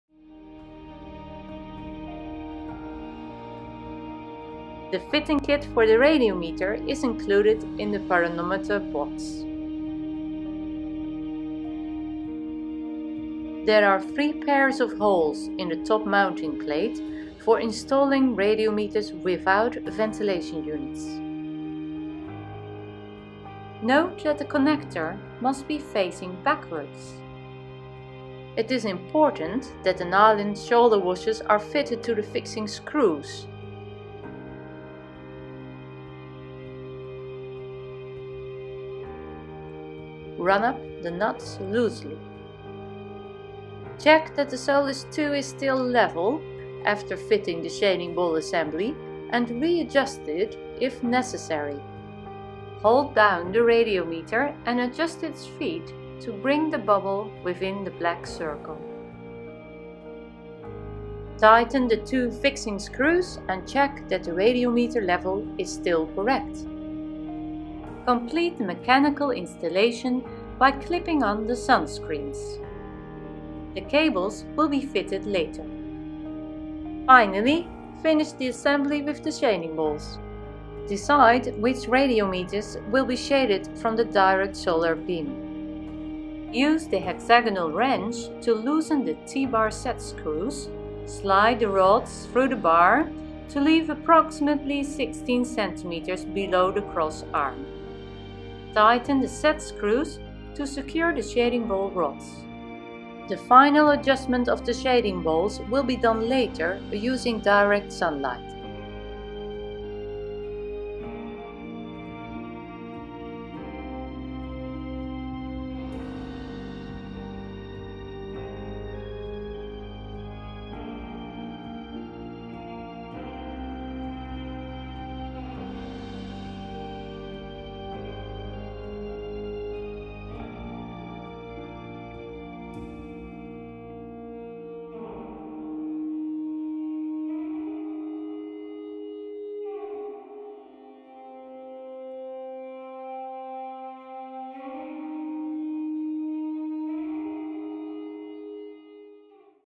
Part 5 - installation of the pyranometer
How to install a pyranometer on the SOLYS 2 sun tracker? This step by step video will demonstrate how easy it is.
To measure Diffuse Horizontal Irradiance (DHI) or Global Horizontal Irradiance (GHI) you need a pyranometer.  This part of the video shows you how to install and level a  pyranometer on the top mounting plate of the SOLYS 2 shading ball assembly and how to initially adjust the shading ball(s).

A Kipp & Zonen SMP11-V pyranometer is shown, but it could be any CMP or SMP model, or a CGR pyrgeometer. The procedure also applies to mounting a pyranometer or pyrgeometer on any near-horizontal mounting surface for measuring GHI.